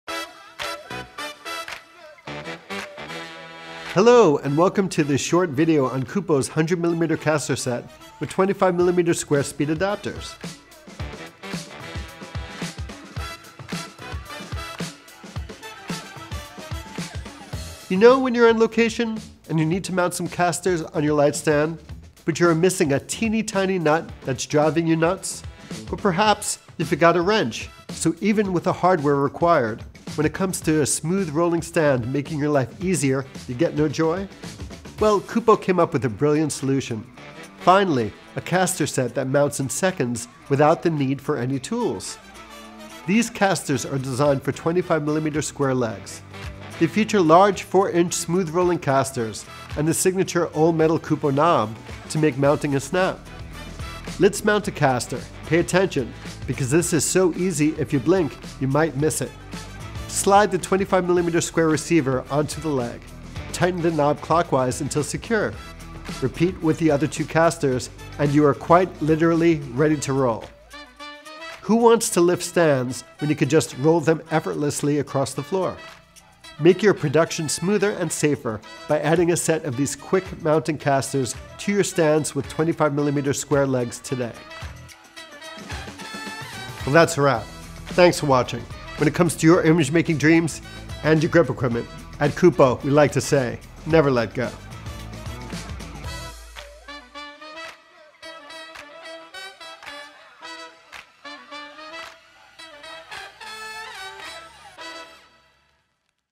Kupo Square Stand Leg Caster Adapter (25mm x 25mm) (Set of 3)
Due to popular demand, Kupo now offers a Square Stand Leg Caster Adapter for the Kupo Slider Stand (KS106111). All Kupo casters are sold in sets of three, and come with brakes to securely lock the stand in place.